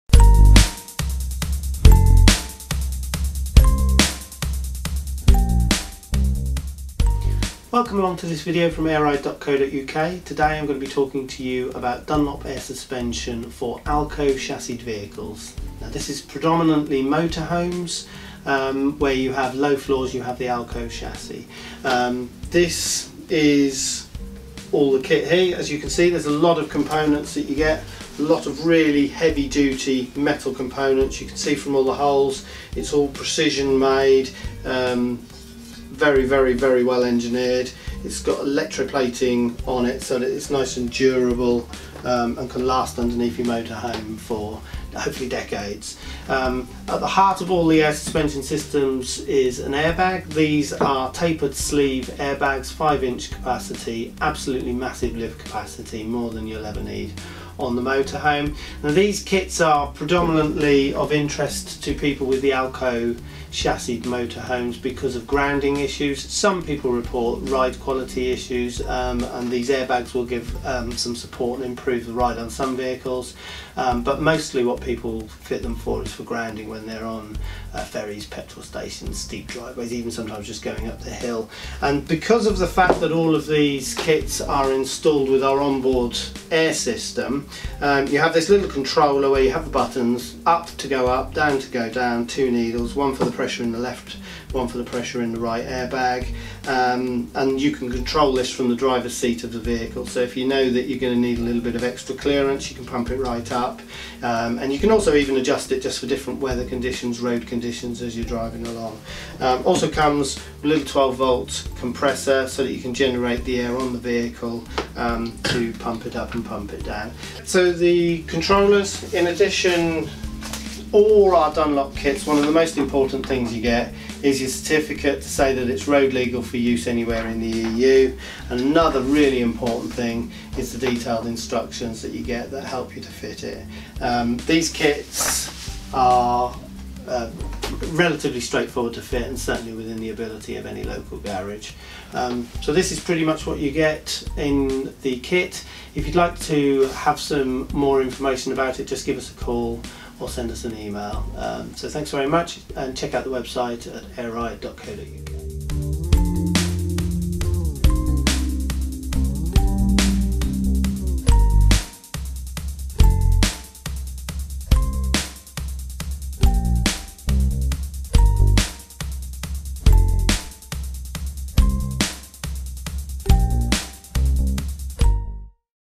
AirRide for AL-KO chassis Motorhome by Dunlop
This kit has now been superseded by a stronger and easier to align kit - details of all kits

The kit in the video is comprehensive and very solid air suspension for the AL-KO chassis often used on motorhomes (and some box vans). The kit is electroplated, uses heavy duty metal and large high quality airbags. Available from AirRide.co.uk this kit provides load support, lifts to overcome grounding issues, and can improve ride and handling. All kits come with OBA (On Board Air supply) which means users can adjust the suspension from the dashboard.